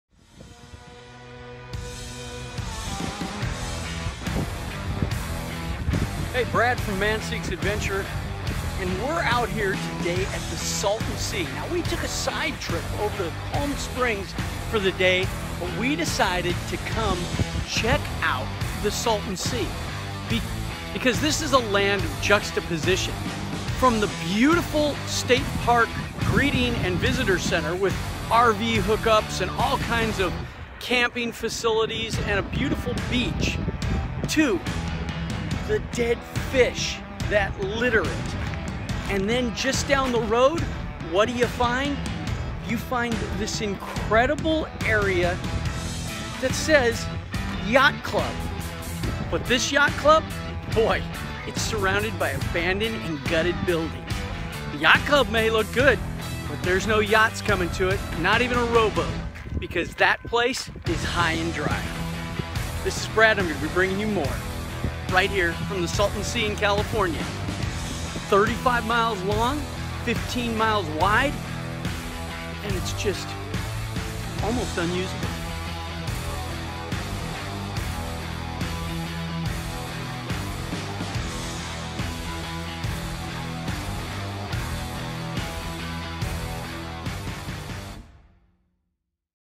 California's Salton Sea State Beach with Man Seeks Adventure
Beach with Brad from Man Seeks Adventure. Walk the ghost beach of California's Salton Sea and hear about how this mistake in the desert has been lost.